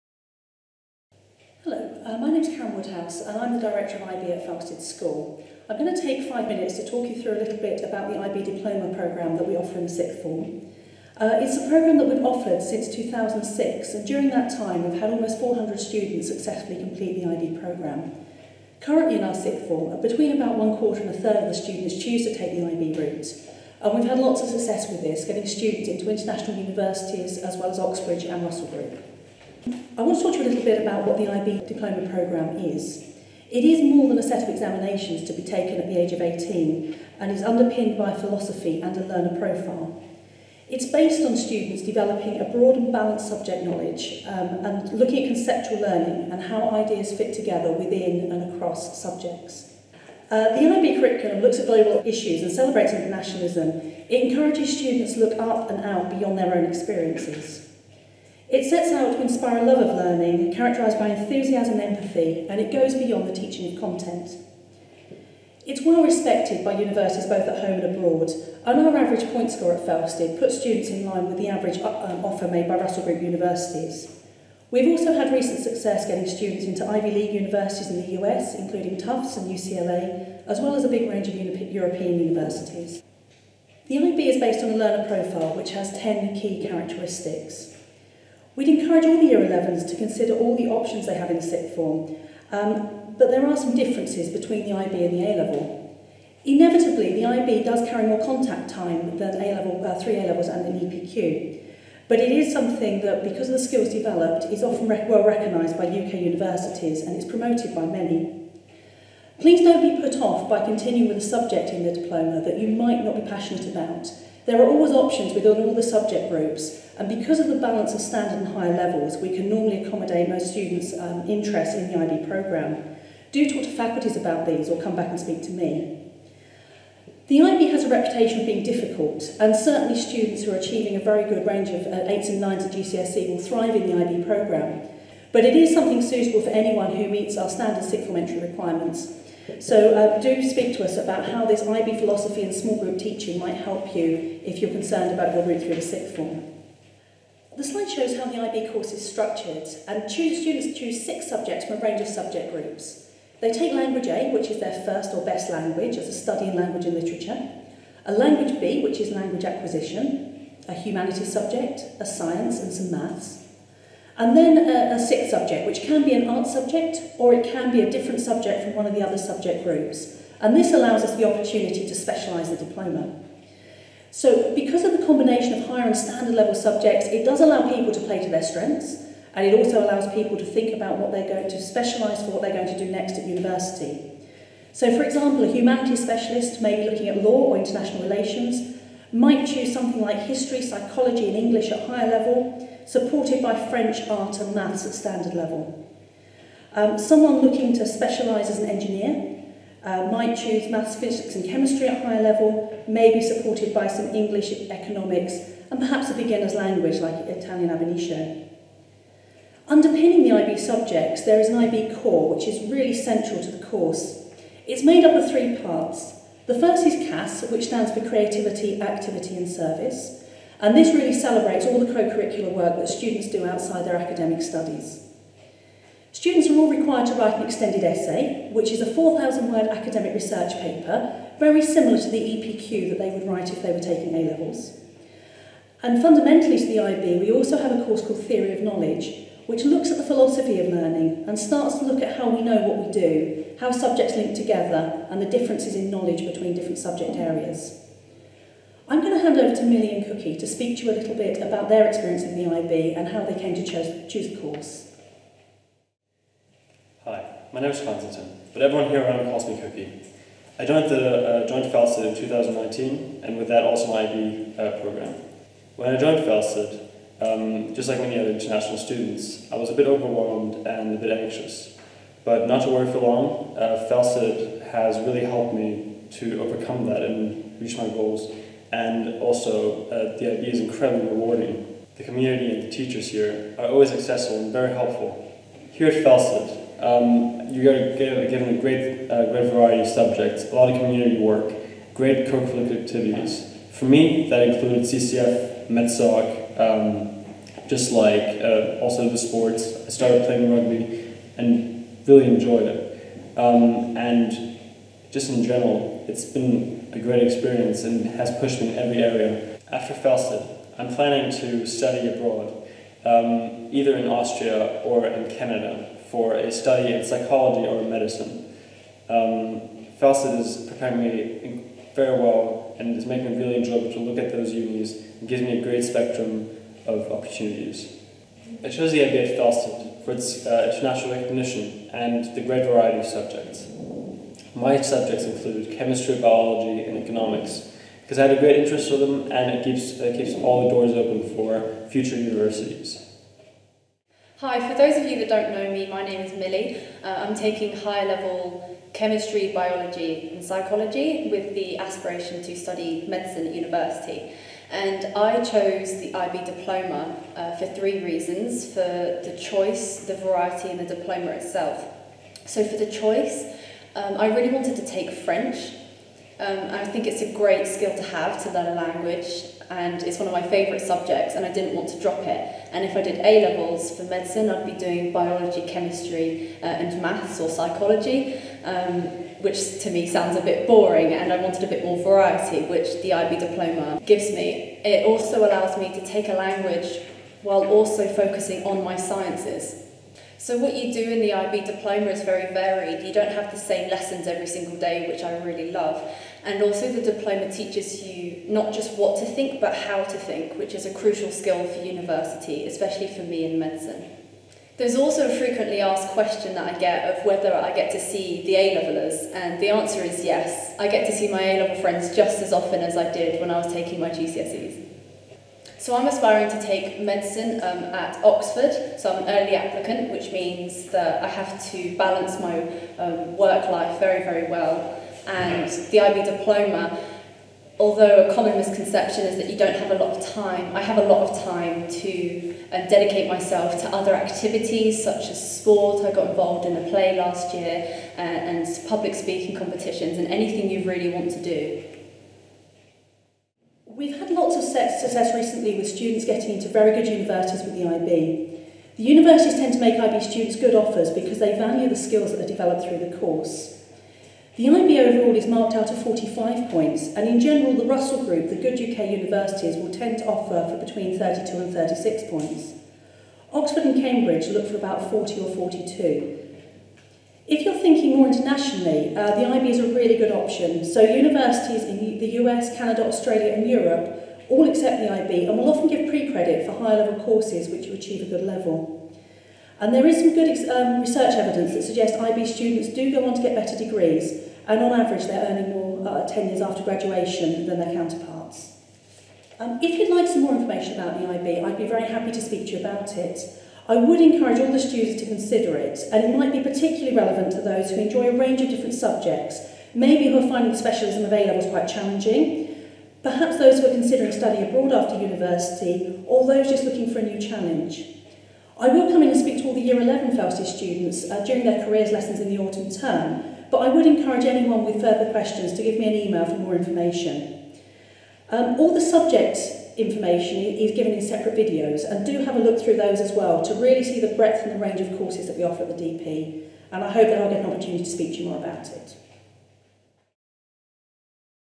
Felsted Sixth Form - IB Diploma Overview
Felsted's Director of IB Karen Woodhouse introduces the Felsted Sixth Form International Baccalaureate Programme as part of our virtual Sixth Form Subject Fair which took place on 4 November 2020.